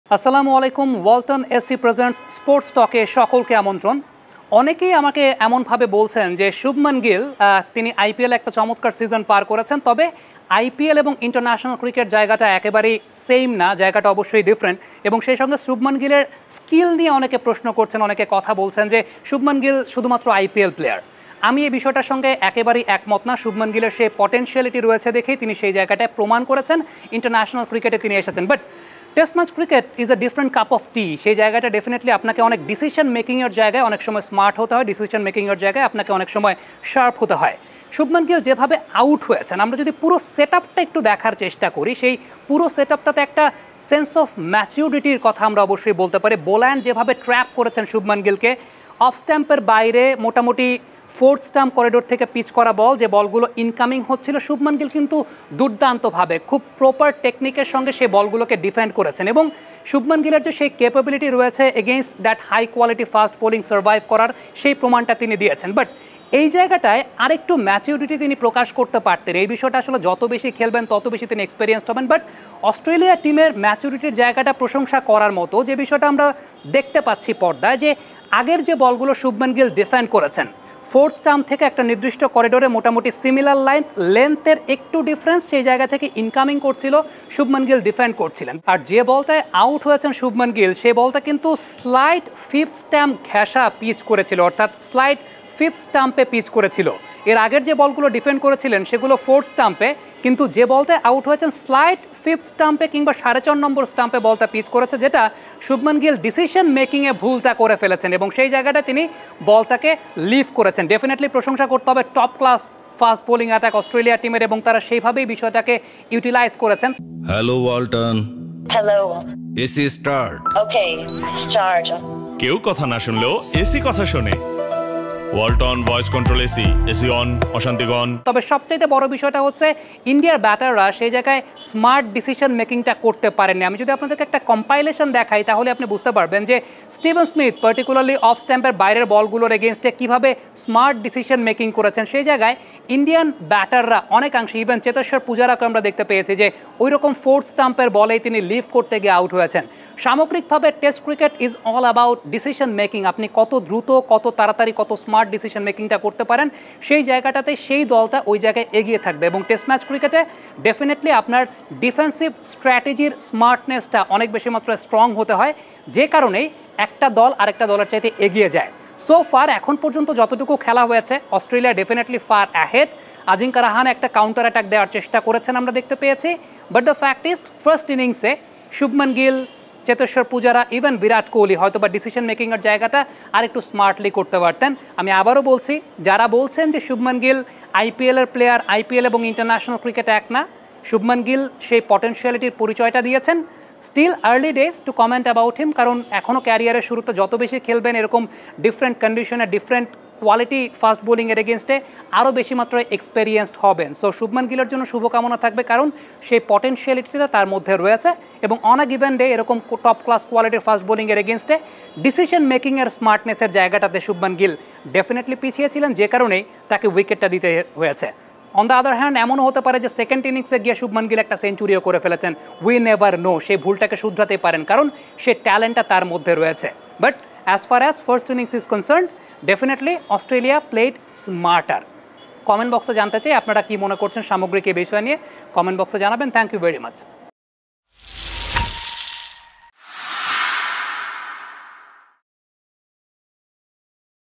শুভমান গিল, IPL আর টেস্ট এক নয়, পার্থক্য বুঝছেন তিনি || On Field 2023
𝐇𝐎𝐒𝐓
𝙎𝙮𝙚𝙙 𝘼𝙗𝙞𝙙 𝙃𝙪𝙨𝙨𝙖𝙞𝙣 𝙎𝙖𝙢𝙞
1st DIV. Cricketer and Cricket Analyst

𝐏𝐫𝐨𝐝𝐮𝐜𝐭𝐢𝐨𝐧
ON FIELD